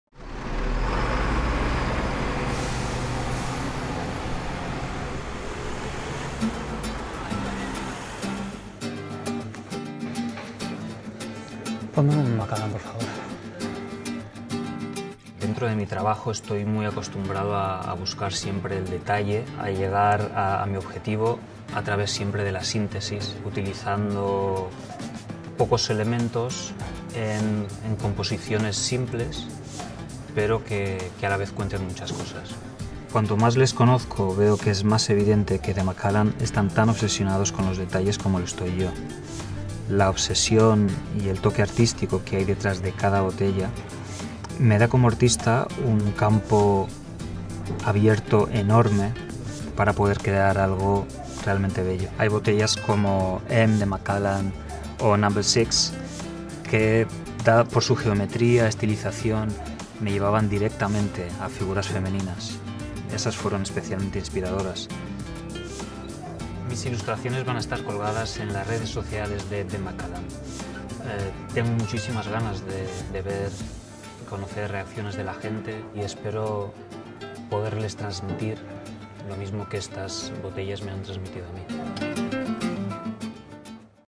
The Macallan and Chidy Wayne Explore Obsessions
Spanish illustrator Chidy Wayne has collaborated with The Macallan to interpret some of our most interesting and iconic whiskies. Using simple lines and watercolour, he tells the story of our whisky through his obsession for beauty and form.